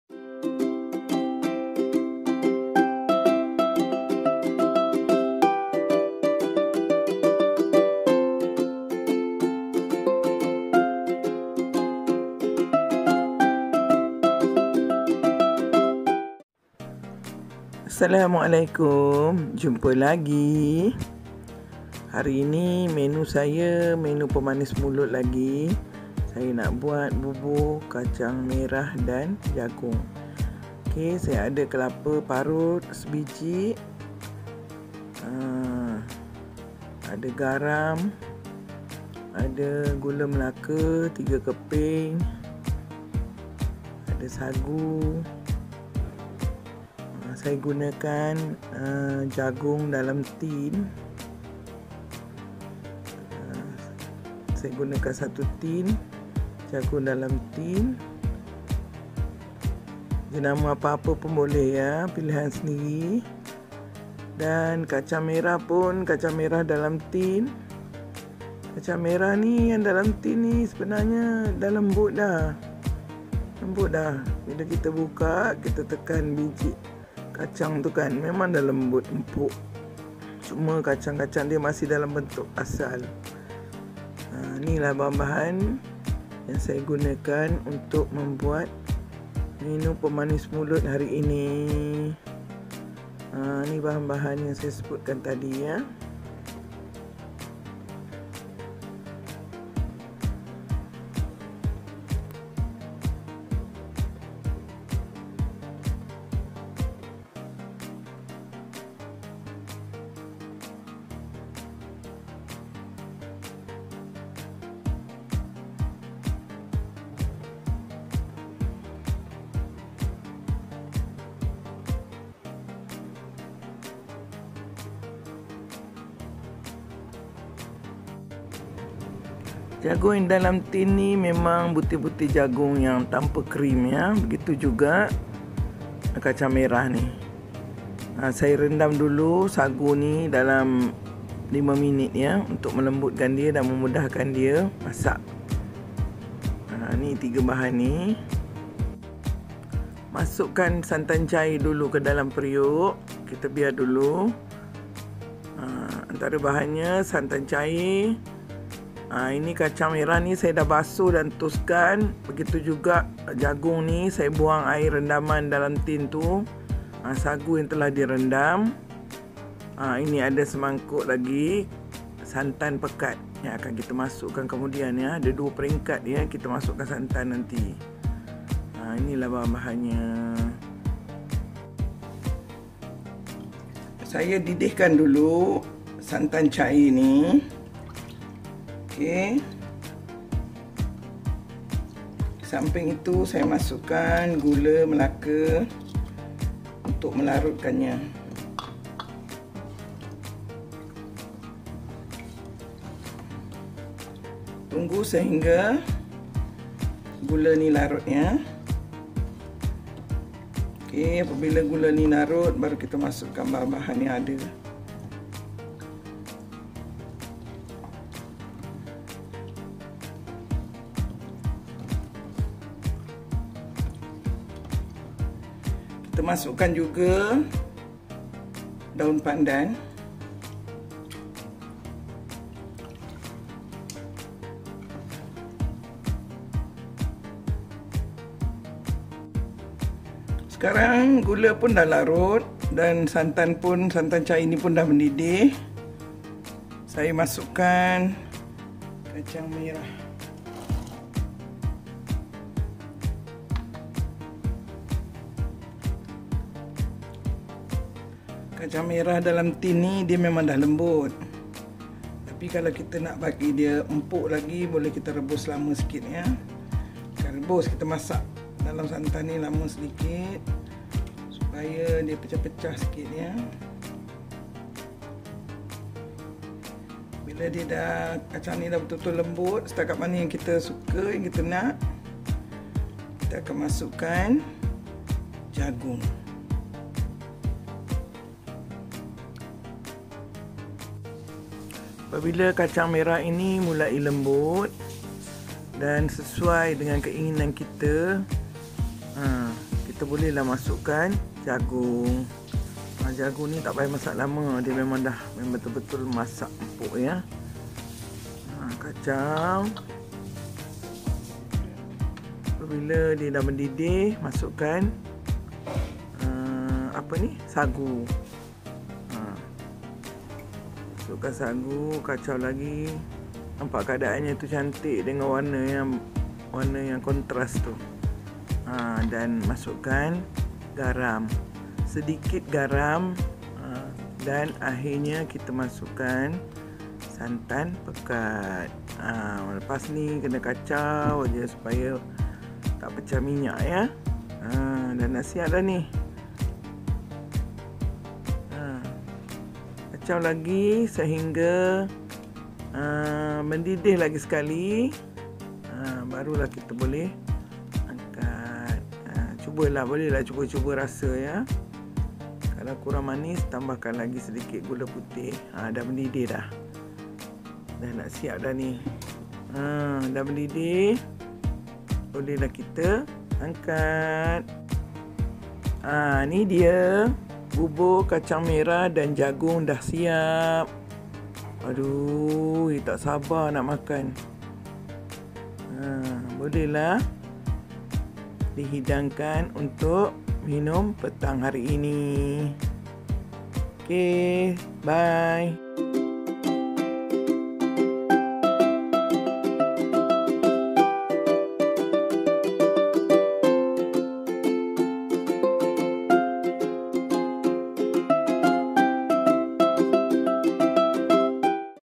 BUBUR KACANG MERAH + JAGUNG ‼️gabungan dua jenis bijirin yang enak ‼️asyik mengunyah isi bubur ini
Juadah pemanisi mulut yang begitu enak dan lazat.